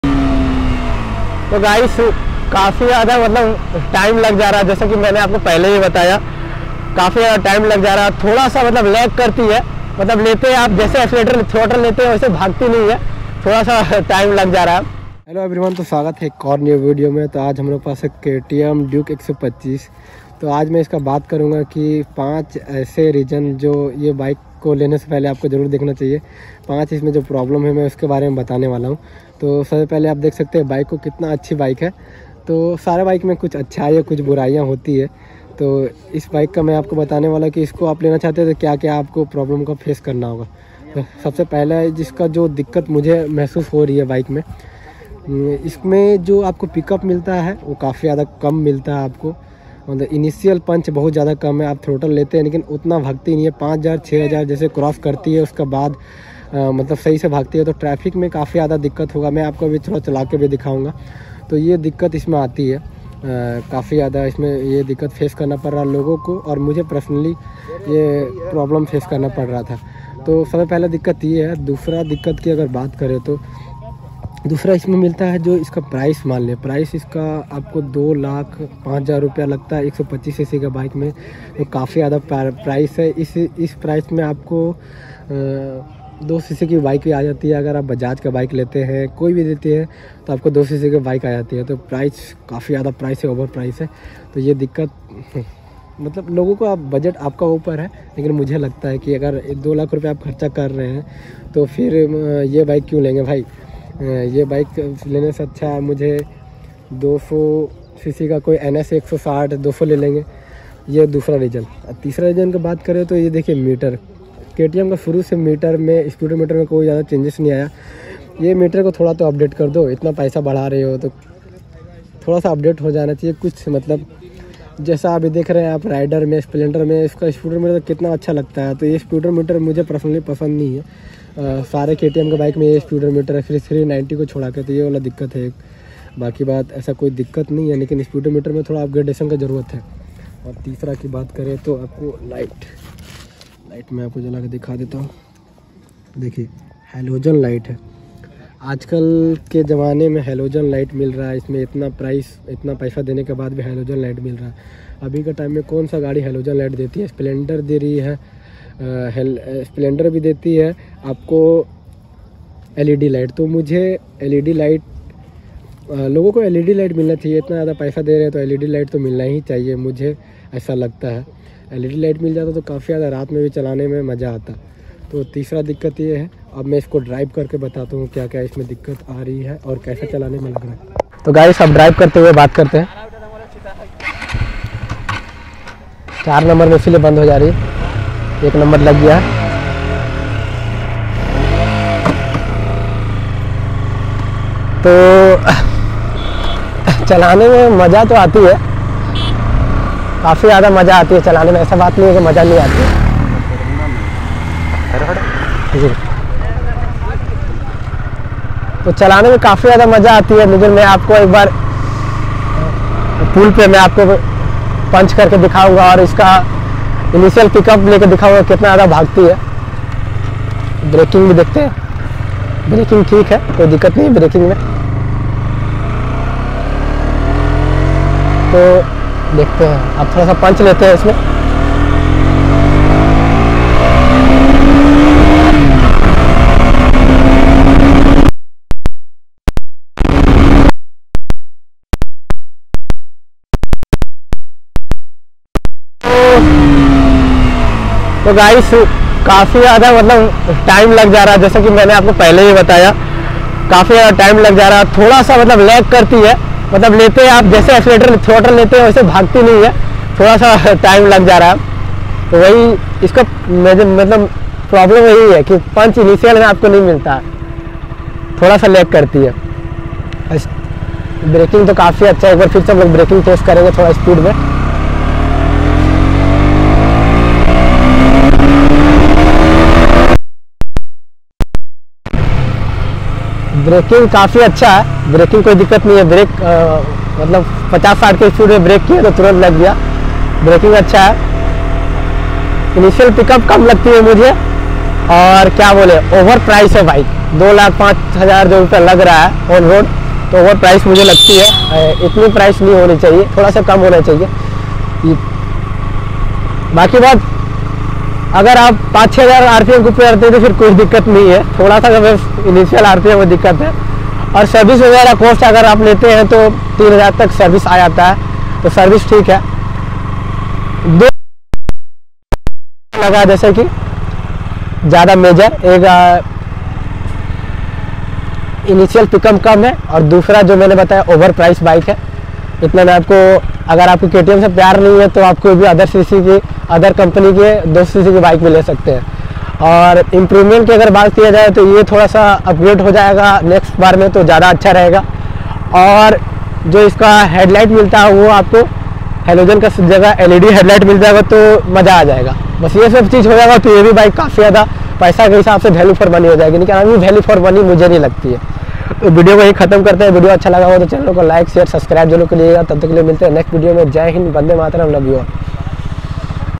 5 Biggest Problem in Ktm duke 125 😡 | 2023 Ktm Duke 125 खरीदने से पहले यह वीडियो जरूर देख ले! ❌
5 Biggest Problem in Ktm duke 125 😡 | 2023 Ktm Duke 125 खरीदने से पहले यह वीडियो जरूर देख ले! ❌